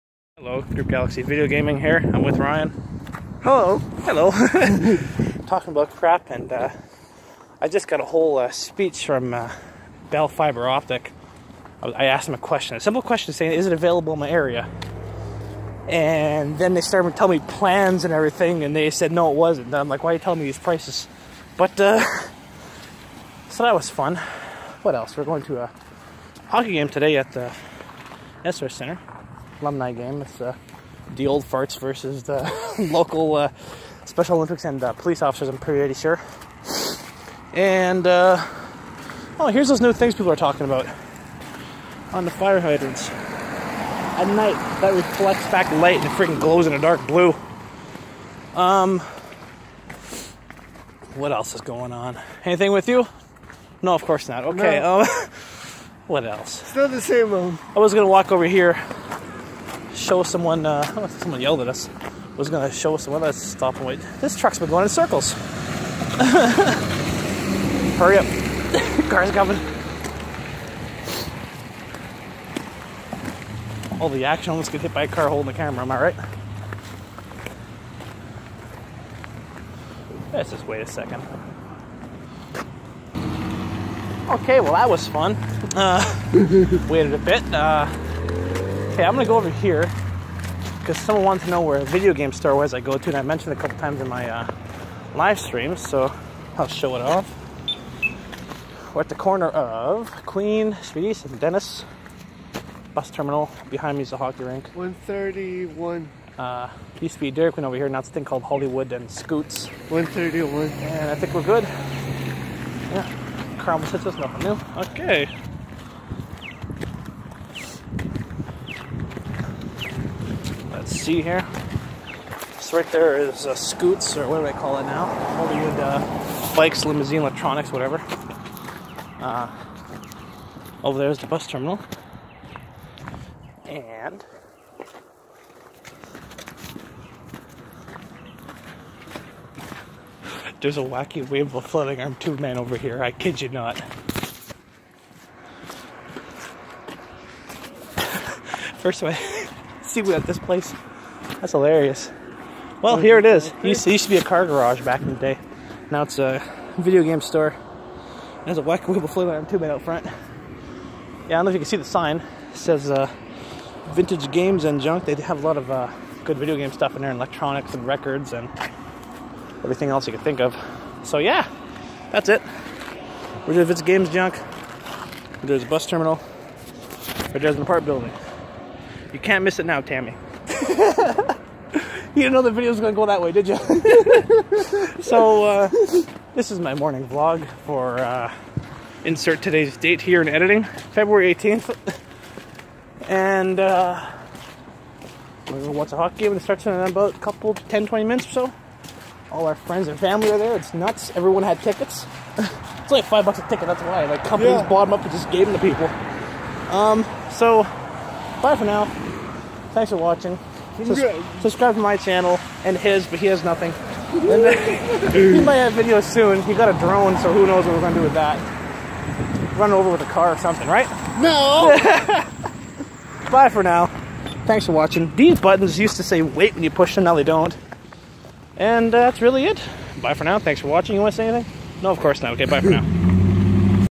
Morning vlog with a friend
Walking to NHL alumni game with big Ryan and talking about random stuff